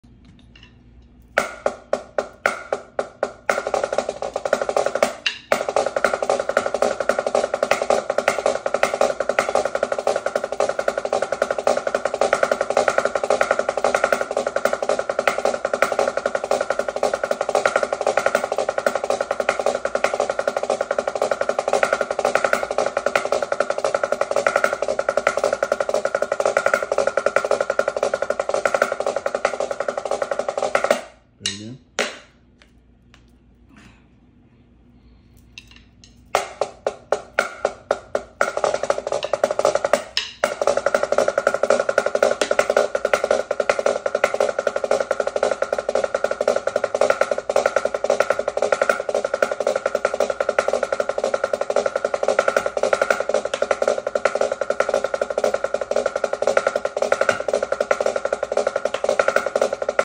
The paradiddle diddle snare rudiment like you've never seen it before
Take the paradiddle - diddle rudiment , move the accent and then add buzzes . That's some cold snare drumming right there by @dessycrew  .